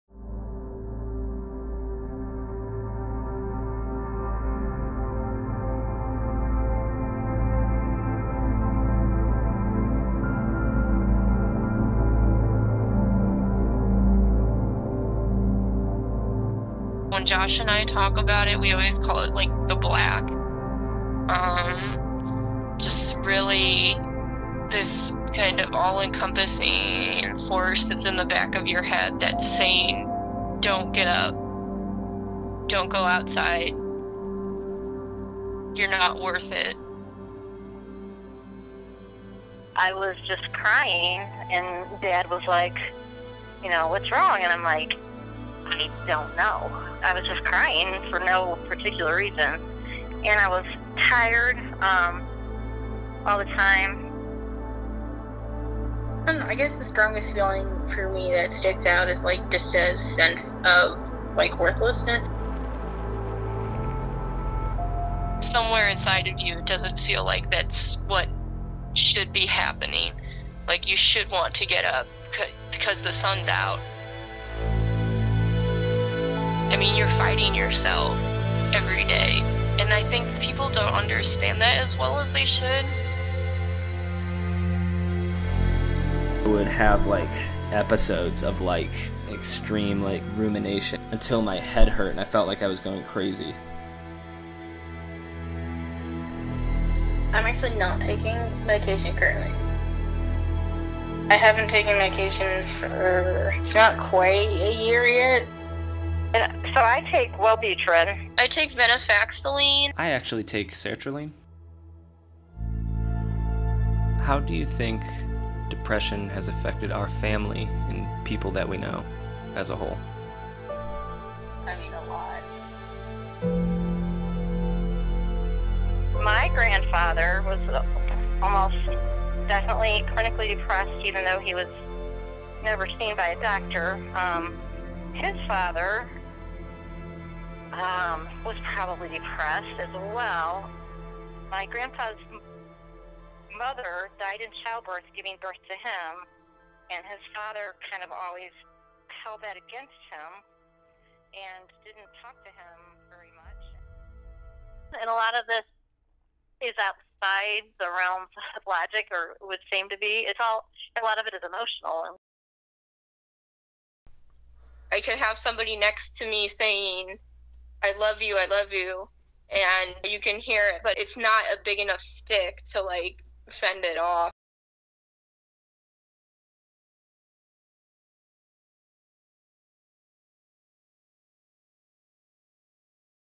In This Season We Suffer
This poetic documentary is a small exploration into what it feels like to have depression, personified by the images presented and the voices of my family. Several members of my family, myself included, suffer from mild to serious depression, and in this film you will feel what that feels like and hear what it sounds like.

Created by Ryan Schiller at University of Missouri in 2018 as part of Campus MovieFest, the world’s largest student film festival.

Cast & Crew:
Hannah Schiller – Participant / Other
Emily Hornbuckle – Participant / Other
Mary Schiller – Participant / Other
Ryan Schiller – Captain, Actor, Director, Director of Photography, Animator, Sound Editor, Editor, Writer
Maggie Schiller – Participant / Other